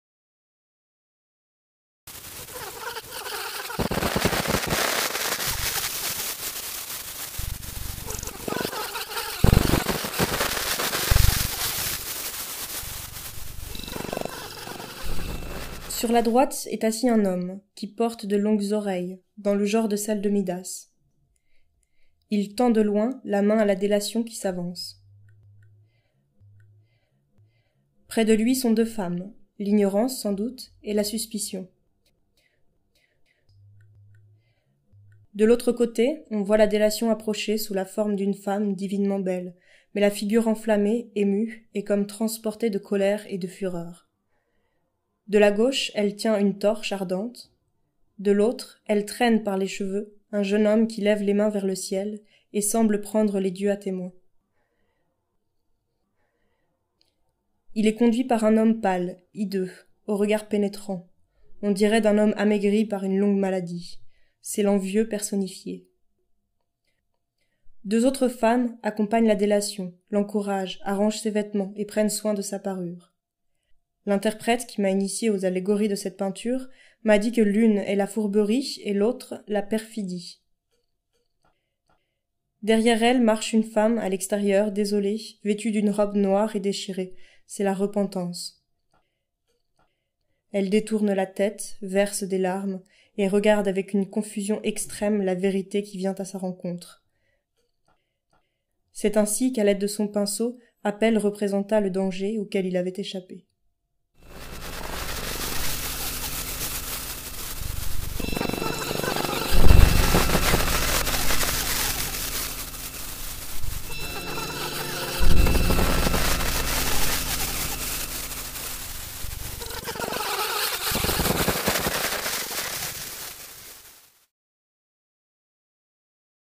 Ekphrasis - Albane Moll (stop motion)
video réalisée en stop motion (papiers découpés) pour le labo collectif du Terrier ) d'après l'Ekphrasis de Lucien de Samosate (la Calomnie d'Apelle)